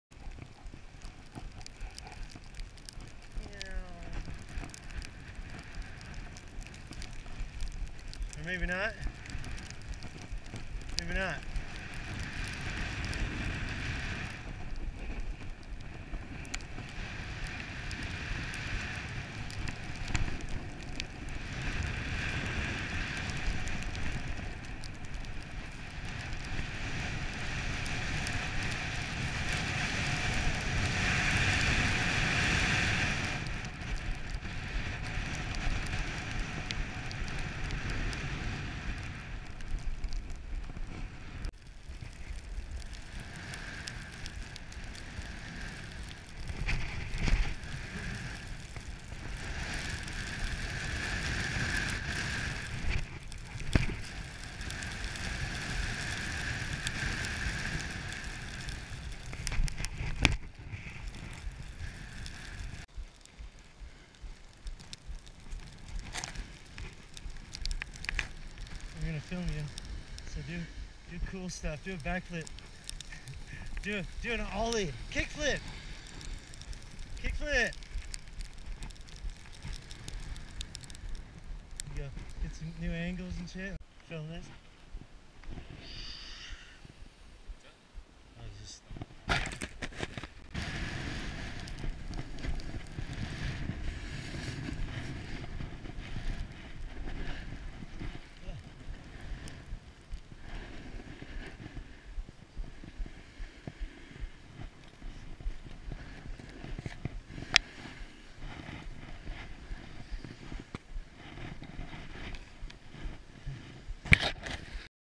Snowboarding at Sierra 1/9/2016
My buddy and I snowboarding at Sierra at Tahoe. Nothing great, just messing around during our second day of snowboarding in many years.